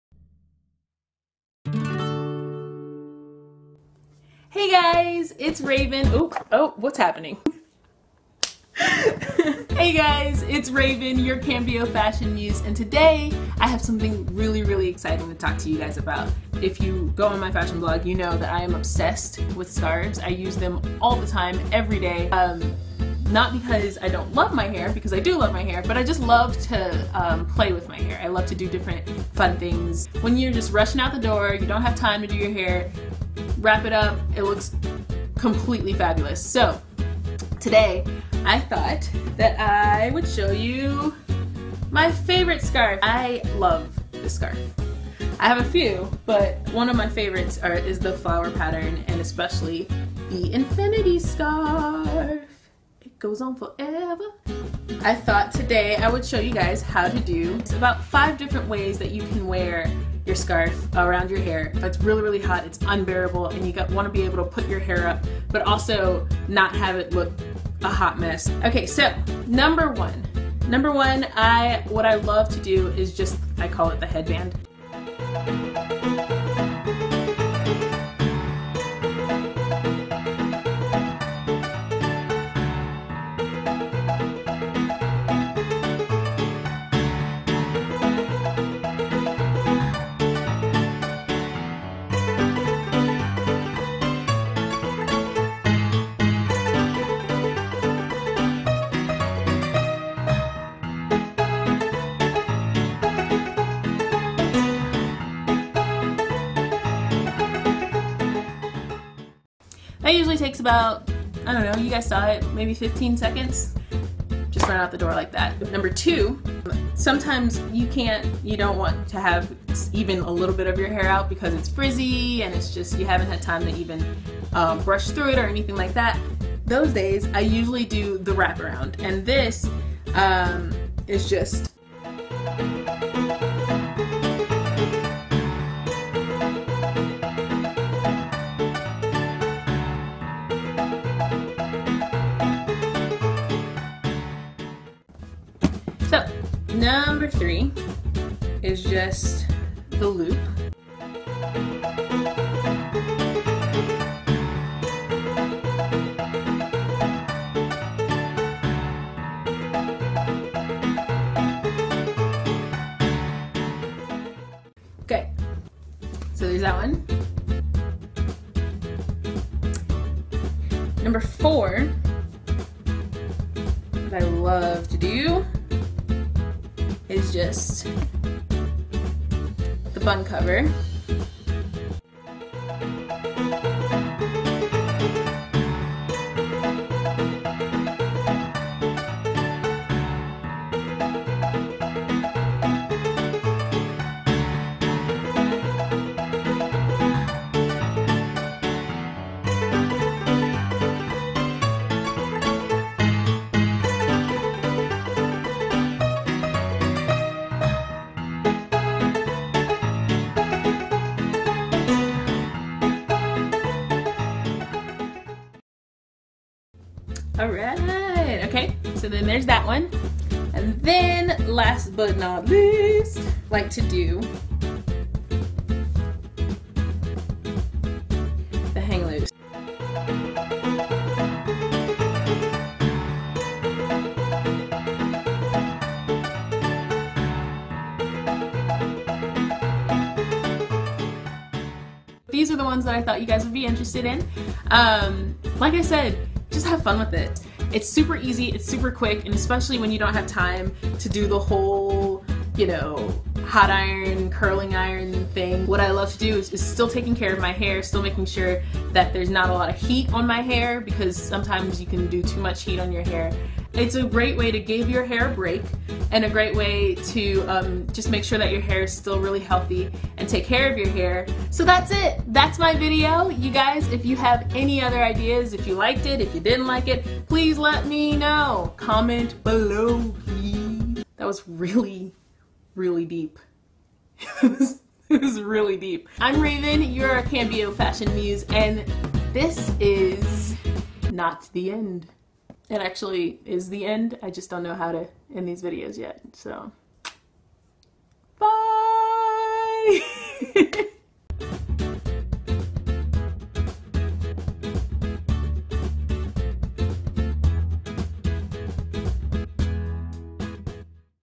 How to Rock a Headscarf
Throwback to one of my first ever videos on 5 different phenomenal ways to rock a headscarf! Let me know what you guys think!